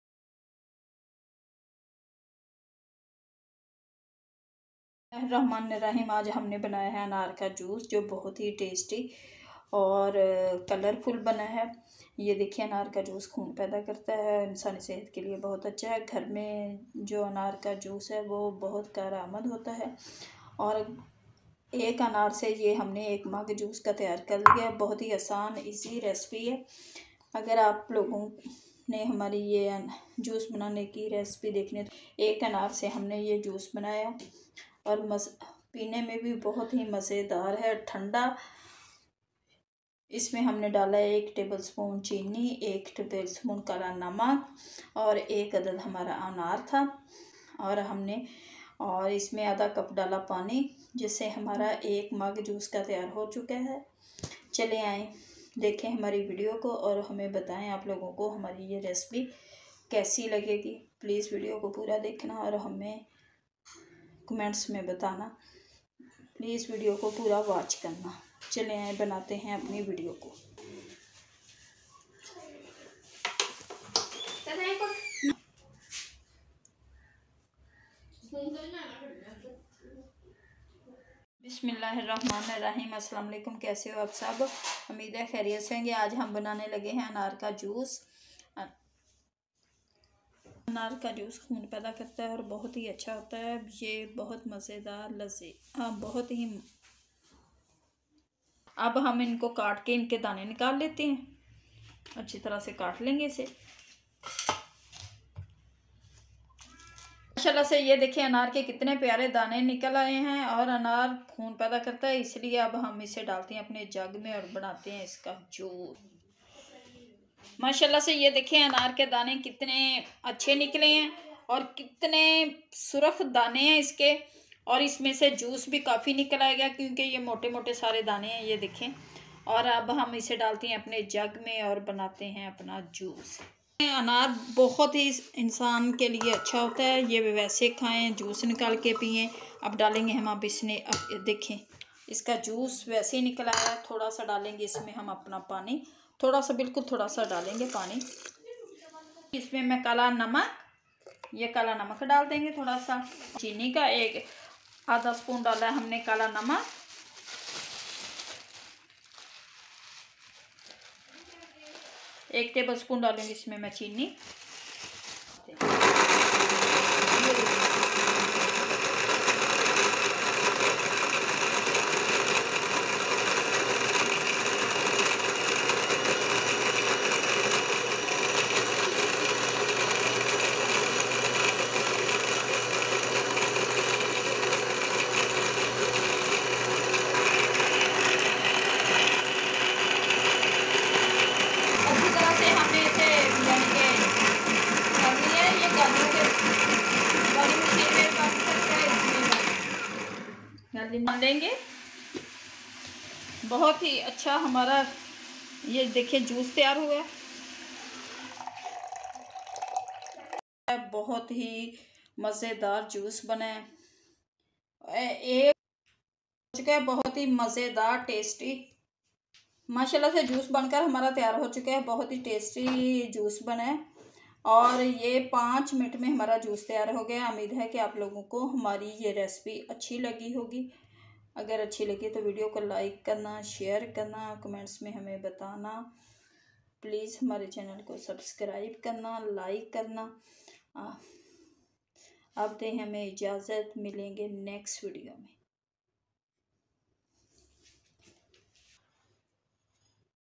Pomegranate juice recipe in pakistan/Anar ka juice recipe/
Aslamualikum

Pomegranate juice recipe in pakistan/Anar ka juice recipe/nasim food secrets

2 anar
Suger
Kala namek
Water

TARKEEB

aj ham banane ja rehe gein anar ka juice jis ke lie ham ne 2 anar lie hein sab se pehle ham anar ko chil ker nekal le ge pher ham blender me dale ge anar pher dale ge kala namek pher thora sa dale ge water blender ko ker de ge on 2 mint blend kere ge anar ka juice ban ke tear ho gea he boht mazedar anar ka juice bna he